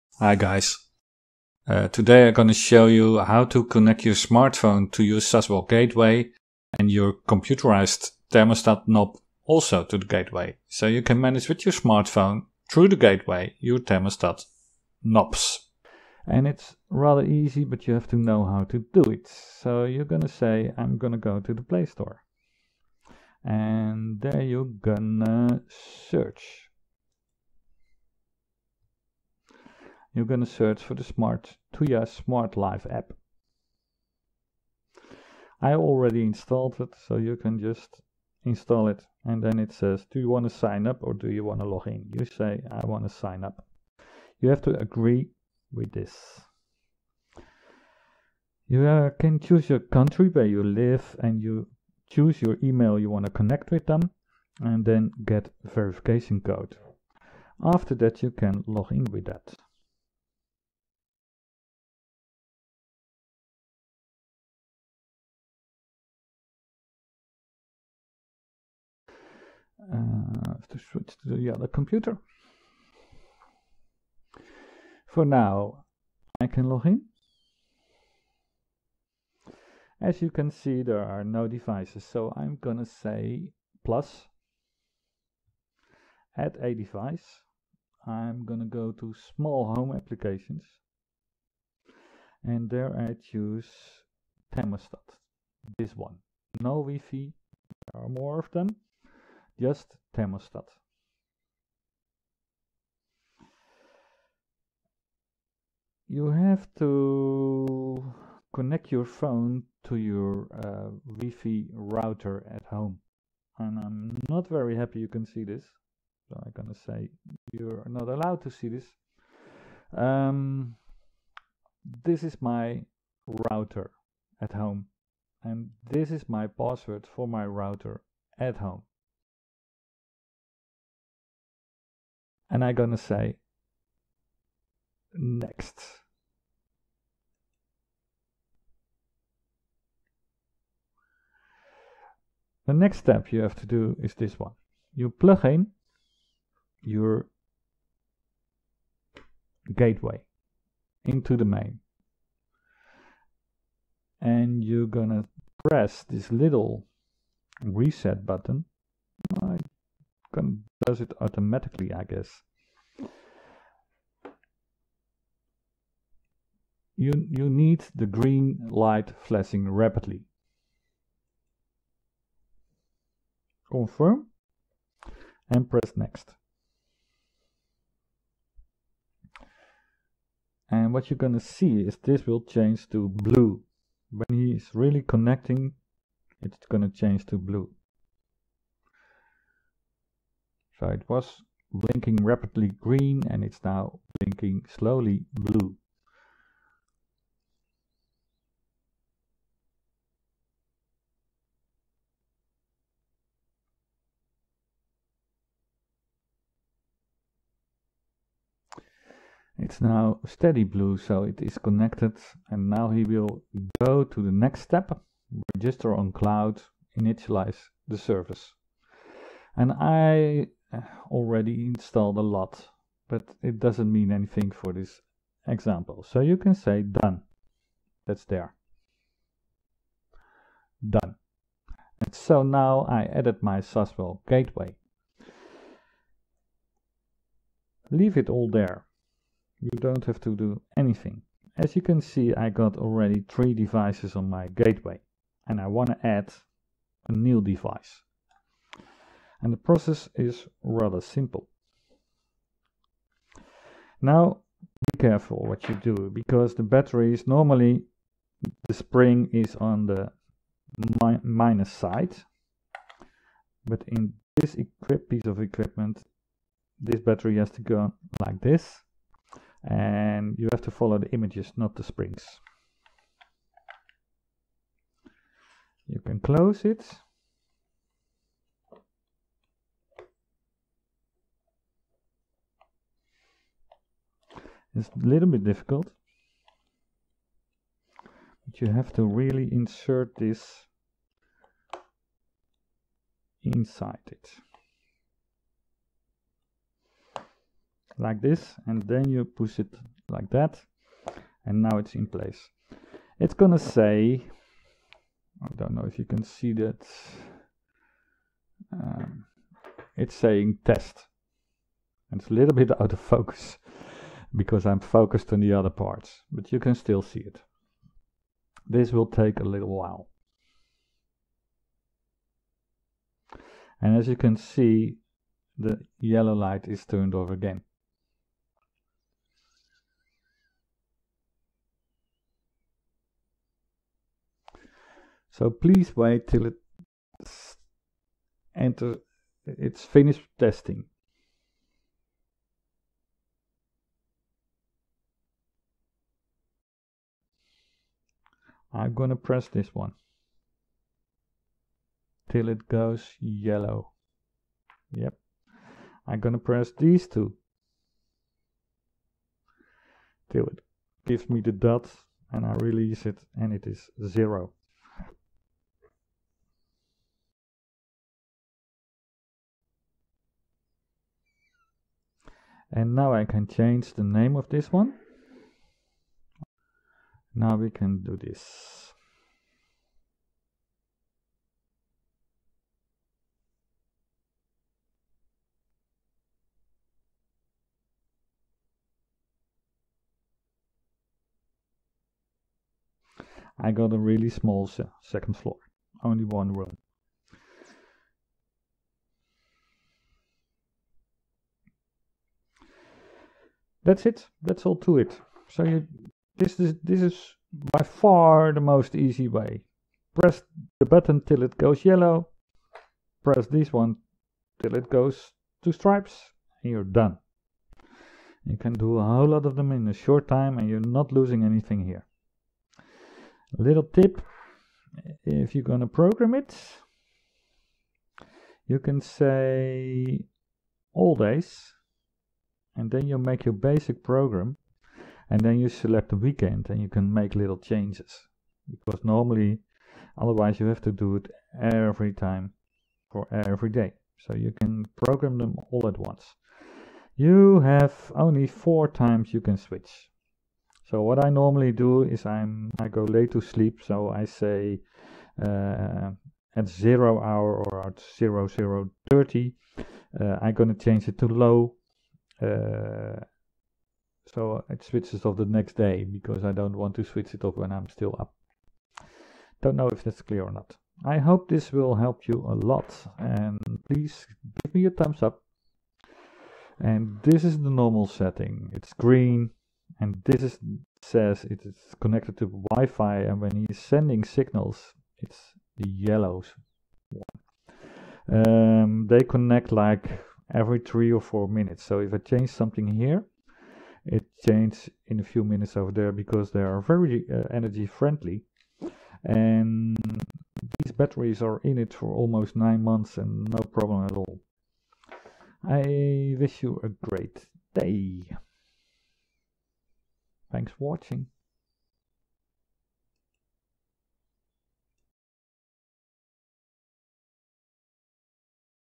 Bind Saswell Saswell thermostatic radiator valve to gateway and smartphone.
This is a Quick howto install the tuya smart home app and connect your phone to the Saswell Gateway SASWG-04-WIFI(TY)
Bind Your Saswell thermostatic radiator valve SEA801-TY to the gateway

4:00 --  Binding a new thermostatic radiator valve starts